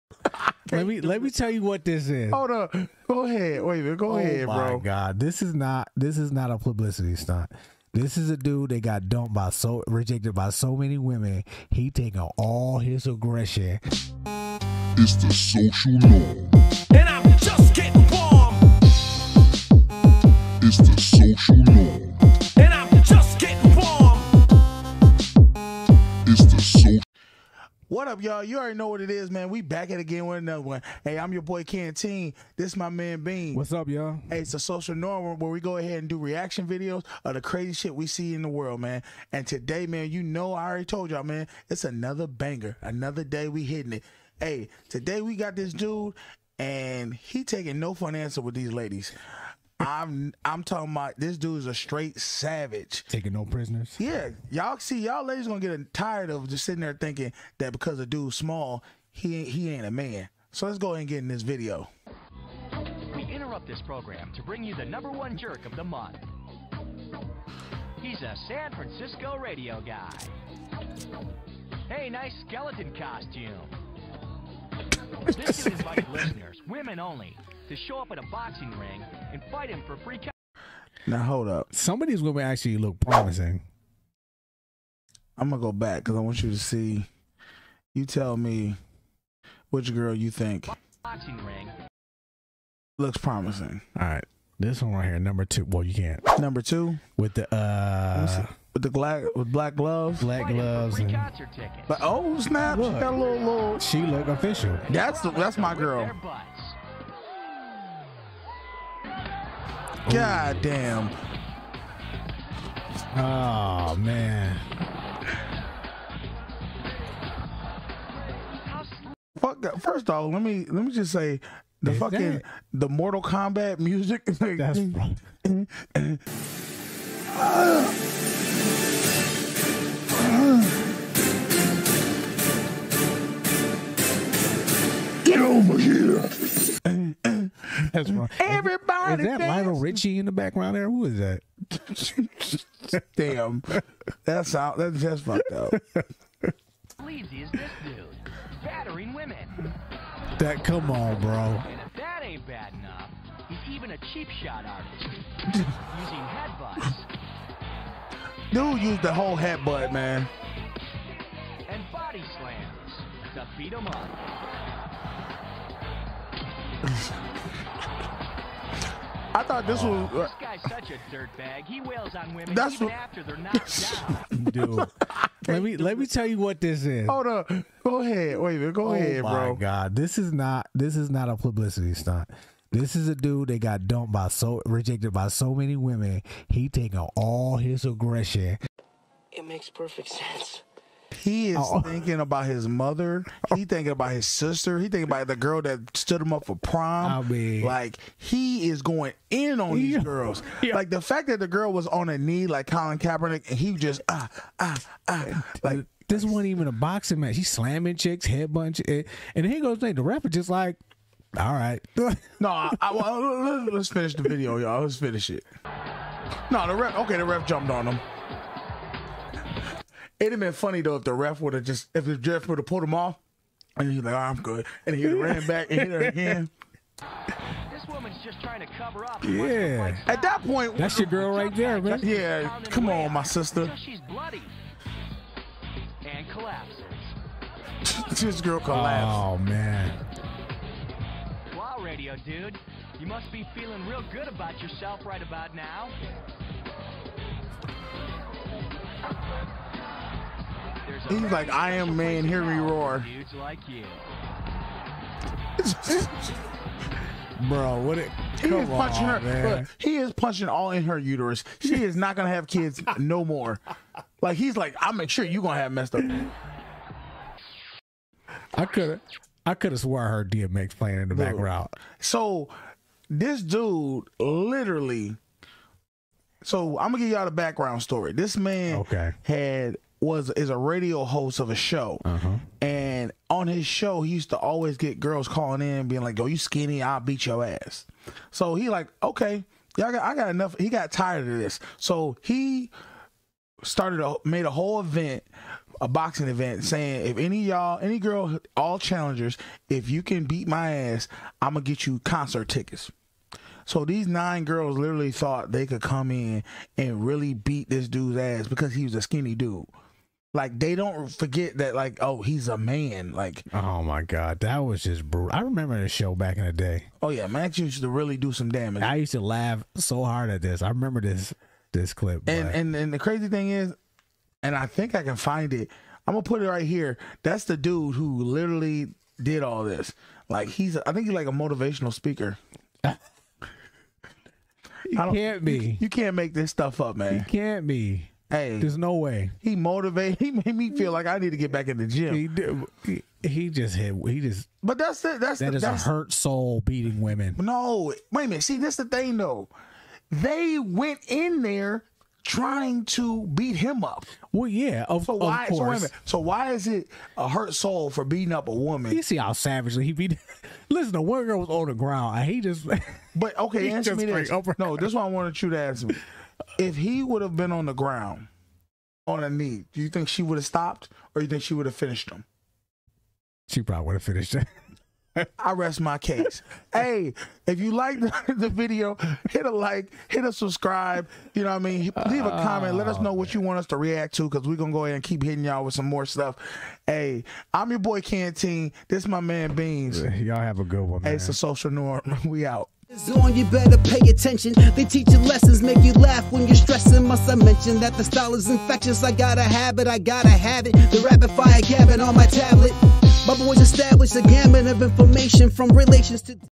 9 Women vs 1 Men
On this video we react to 9 Women vs 1 Men. Today on The Social Norm we see what happens when a little man has enough of women saying they can beat him up... You wont believe what happens!